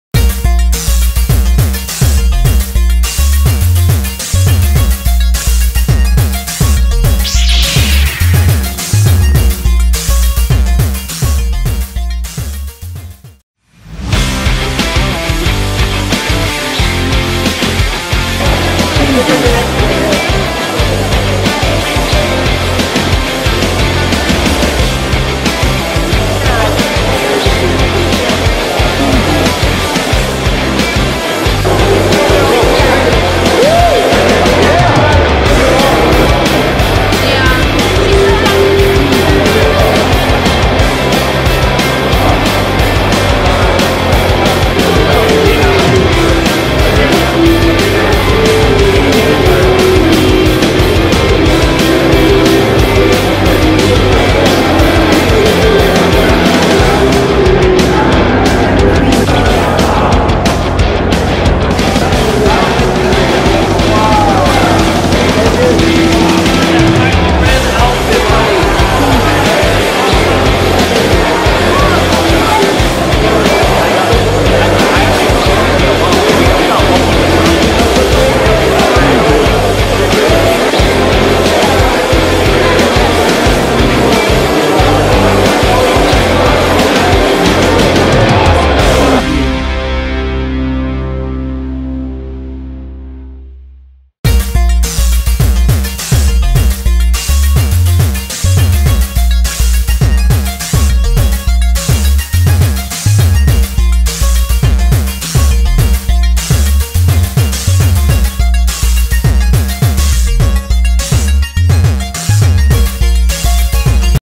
RICC 2019 Cosplay 2
RICC had a lot to offer in the way of awesome cosplayers. Huge variety of fandoms represented and some really talented people behind it. So much cosplay we couldn't contain it all in one video!